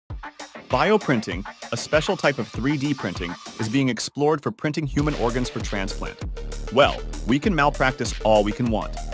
They are 3d printing .. what now?
In this groundbreaking video, we delve deep into the innovative world of 3D printing and its transformative impact on the medical field, specifically in the creation of artificial organs for transplantation. As the demand for organ transplants surpasses the supply, 3D printing emerges as a beacon of hope, offering groundbreaking solutions to save lives. Join us as we explore how cutting-edge technology is used to design and produce artificial organs that mimic natural functionality, promising a future where organ shortages are a thing of the past.

What You'll Learn:

The Basics of 3D Printing in Medicine: Understand the fundamentals of how 3D printing technology works in creating highly detailed and functional artificial organs.
Breakthroughs in Artificial Organs: Discover the latest advancements and success stories in the field, from kidneys to hearts, and how they're changing patients' lives.
The Future of Organ Transplants: Get insights into the potential future developments in 3D printed organs and how they could revolutionize the transplant process.
Ethical and Technical Challenges: Explore the ethical considerations and technical hurdles that come with artificial organ creation and how the medical community is addressing them.
Whether you're a medical professional, a technology enthusiast, or someone curious about the future of healthcare, this video offers a comprehensive overview of one of the most exciting advancements in modern medicine. Don't miss out on learning about how 3D printing is setting the stage for a new era in organ transplantation.